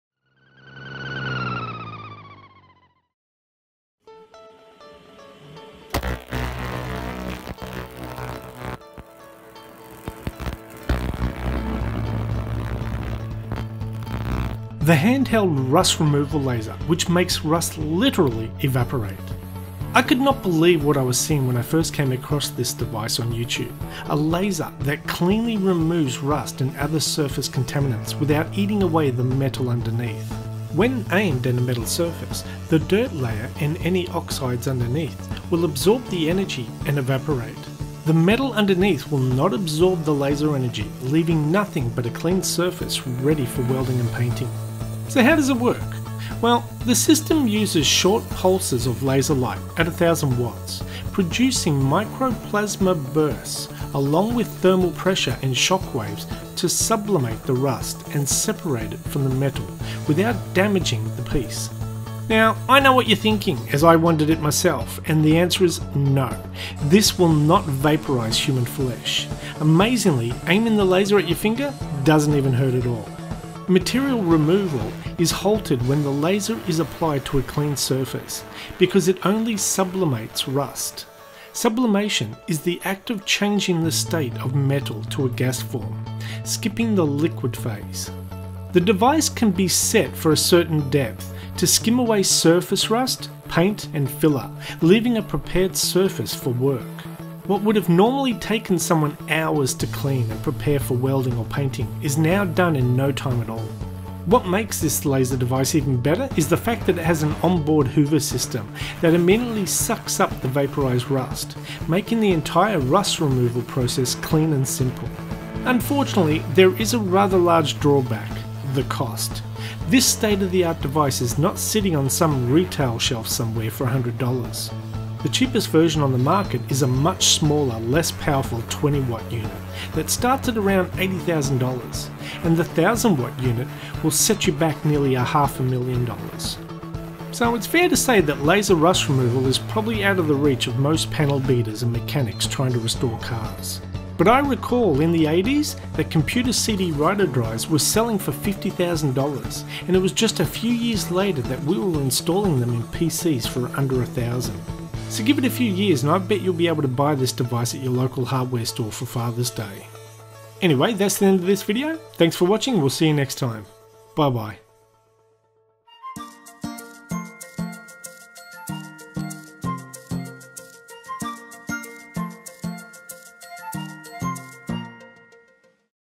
1000W Rust Cleaning Laser - Removes Rust Effortlessly
Rust Cleaning? Remove rust easily from metals using a 1000 Watt Laser. When aimed at a metal surface, the dirt layer and any oxides underneath will absorb the energy and evaporate.

The metal underneath will not absorb the laser energy, leaving nothing but a clean surface ready for welding or painting.

The system uses short pulses of laser light at 1000 watts, producing micro-plasma bursts, along with thermal pressure and shock waves to sublimate the rust and separates it from the metal without damaging the piece.

Here is a basic Price guide in US Dollars

– Low Power (50W – 100W) : $68,000 - $105,000
– Mid Power (200W – 500W) : $160,000 - $295,000
– High Power (1000W) : $440,000 - $500,000

The Hand Held Rust Removal Laser makes rust literally evaporate.

NOTE: This Laser does not harm or hurt human flesh, as seen in the video.

CREDIT LINKS

Here are a few websites that sell the hand held Lasers.